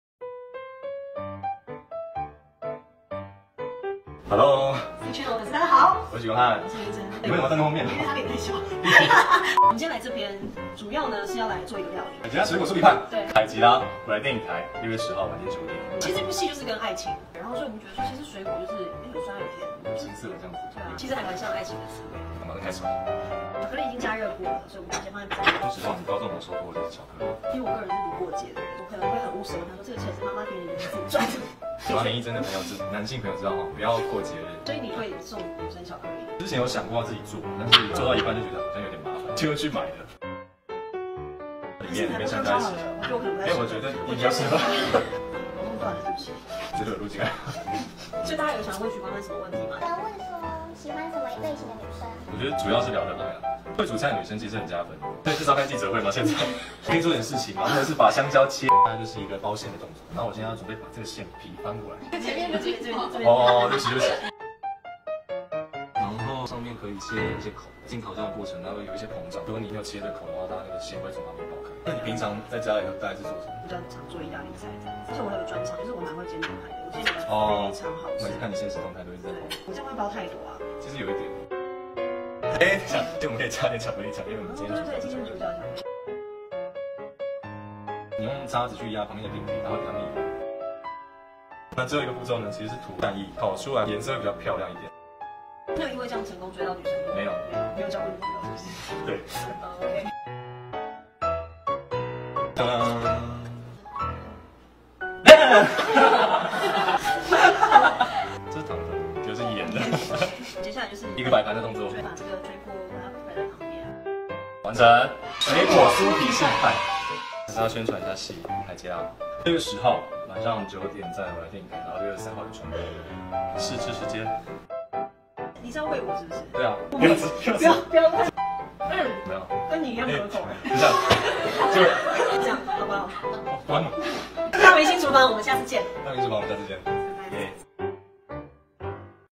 許光漢宣傳"海吉拉" 和林意箴打鬧完成"海吉拉水果酥皮派"!! 20180621
許光漢讓人心動~~~
超可愛的他們XDDDDD